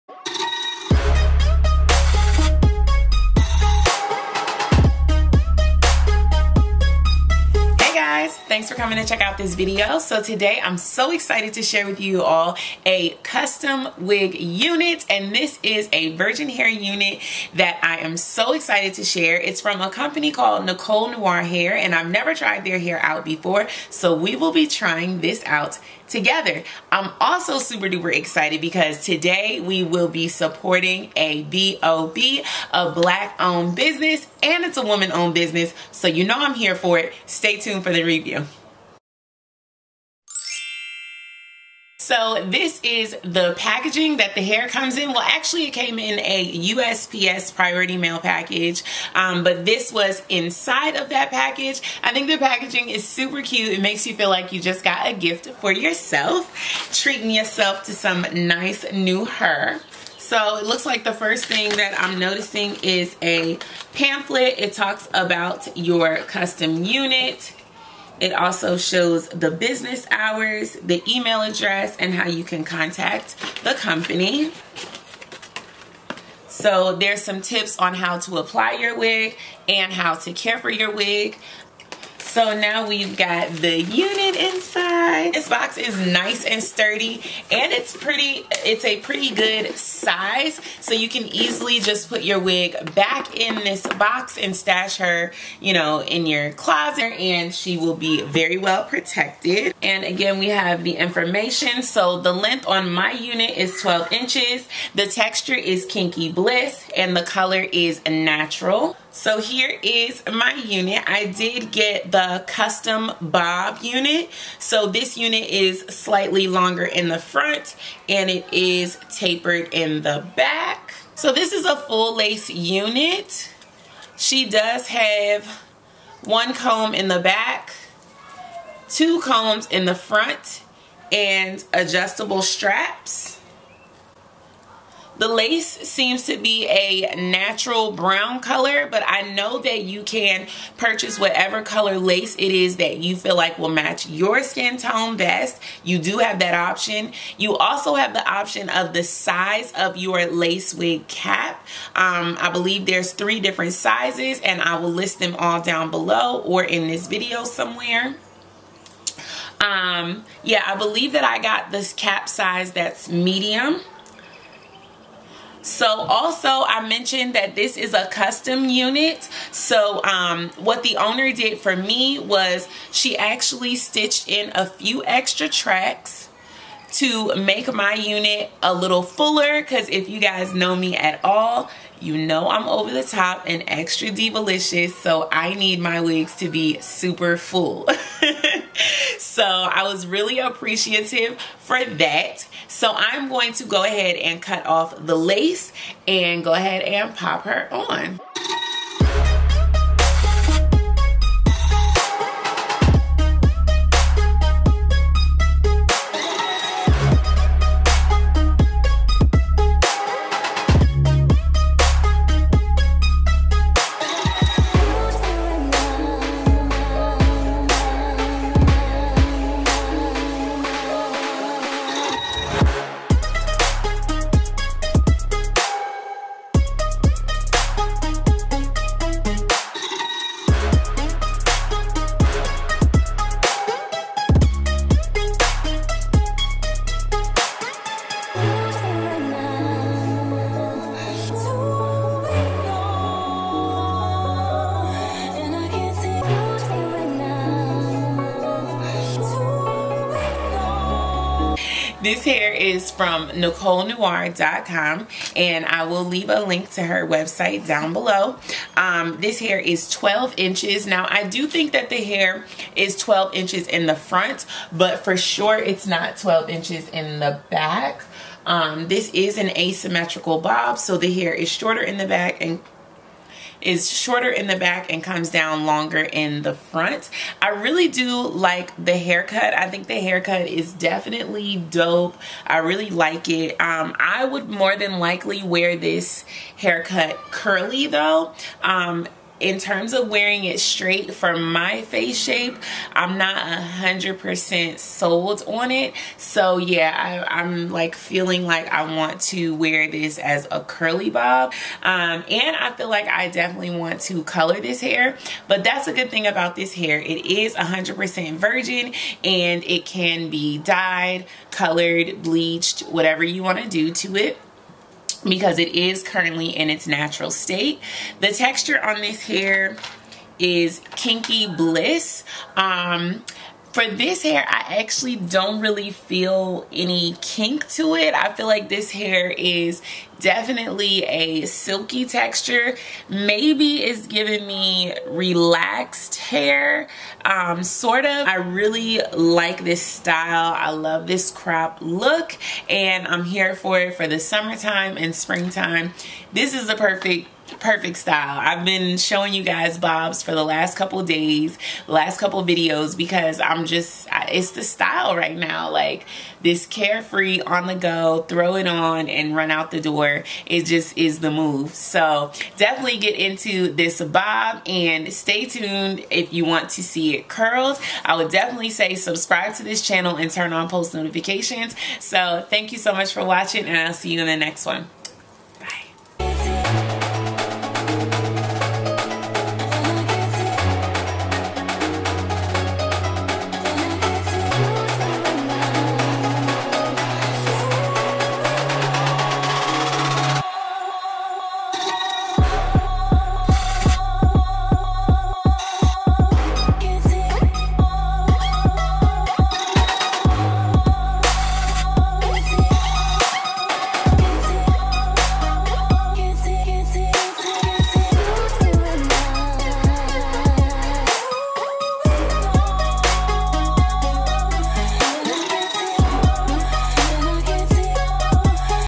BOB SEASON ft. Nicole Noire Hair Kinky Bliss Virgin Hair Custom Made Bob
PLEASE NOTE: THIS HAIR IS NOT IN ITS NATURAL STATE, IT HAS BEEN FLAT IRONED STRAIGHT, AND WILL REVERT BACK TO A KINKY TEXTURE WHEN WASHED.